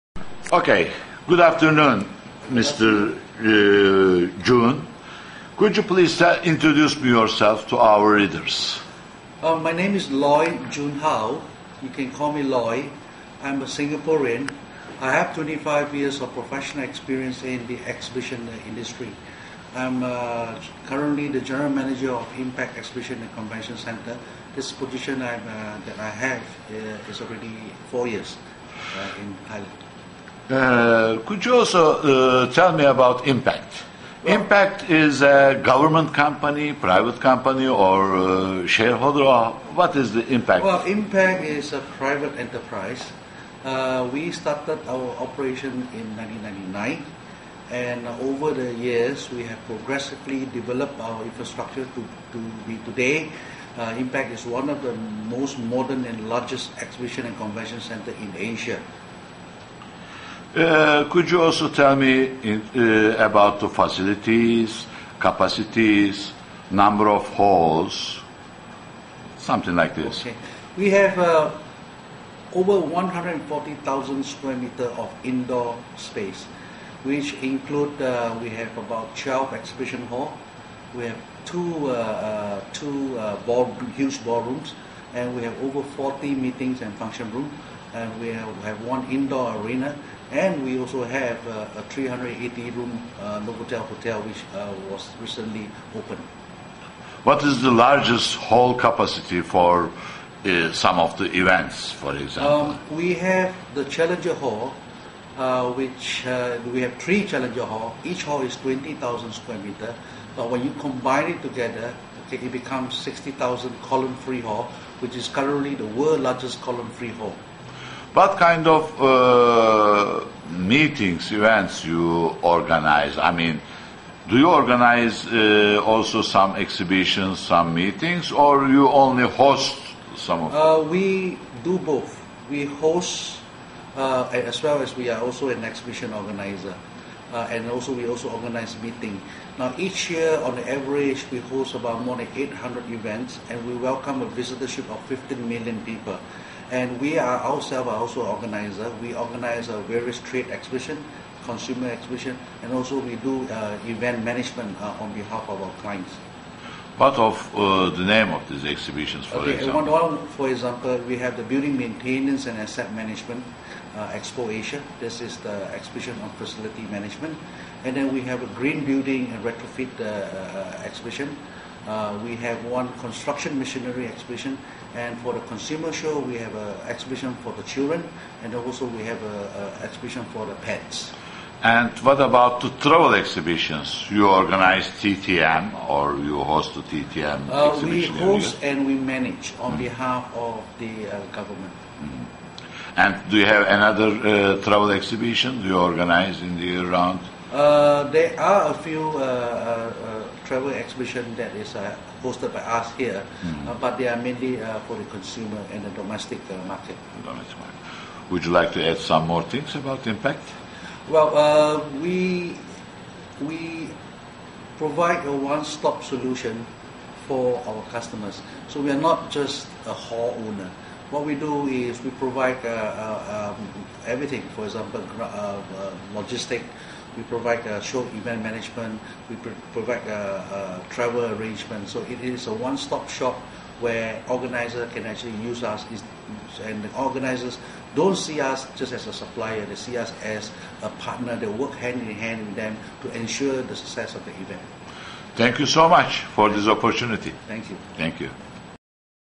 Mr Loy Joon How - IMPACT Thailand
During TTM Plus 2011, FTNnews.com asked Mr Loy Joon How, the General Manager of IMPACT, about his company and their products and services for the meetings industry.